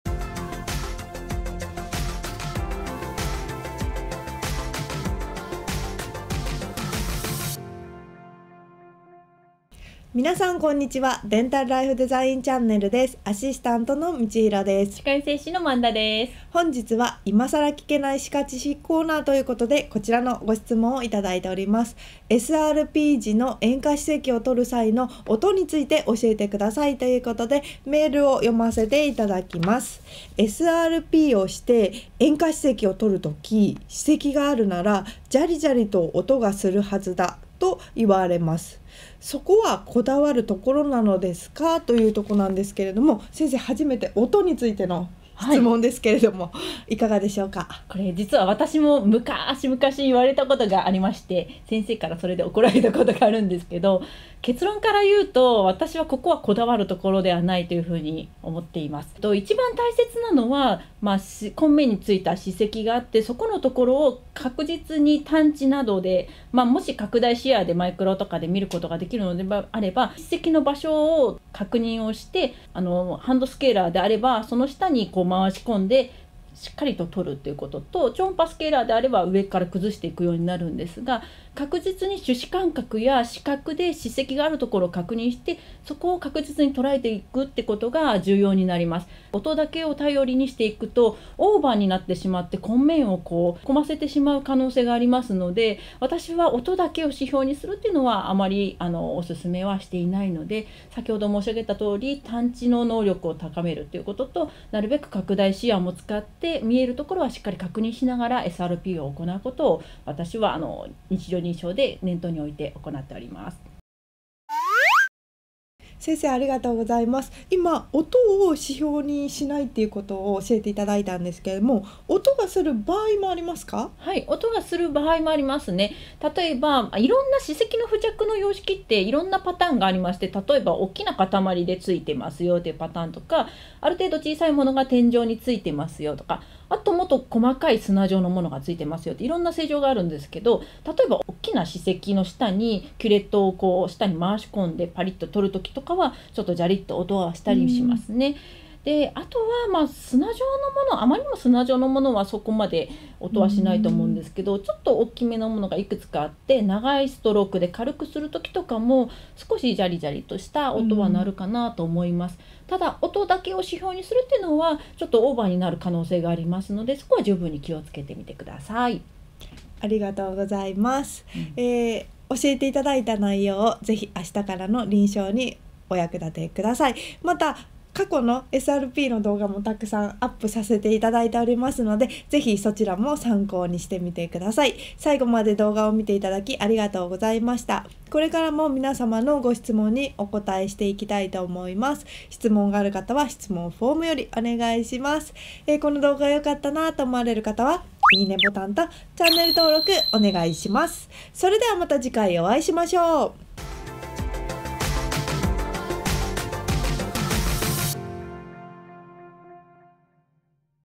【SRPの時の縁下歯石を取る際の音について教えてください】今更聞けない歯科知識 新人歯科衛生士さんのためのお悩み相談室/萬田久美子先生
現役の歯科衛生士さんから頂いたお悩みや疑問に歯科衛生士の萬田久美子先生が回答するお悩み解決動画です。

【動画の内容】
・SRPの時の縁下歯石を取る際の音について歯科衛生士の萬田久美子先生にわかりやすく解説頂いています。

■萬田久美子先生へのお悩み事の相談はこちら

■Dental Life Design　Twitterアカウントのご案内

セミナー、イベント、新着情報などお得な情報をお届けします！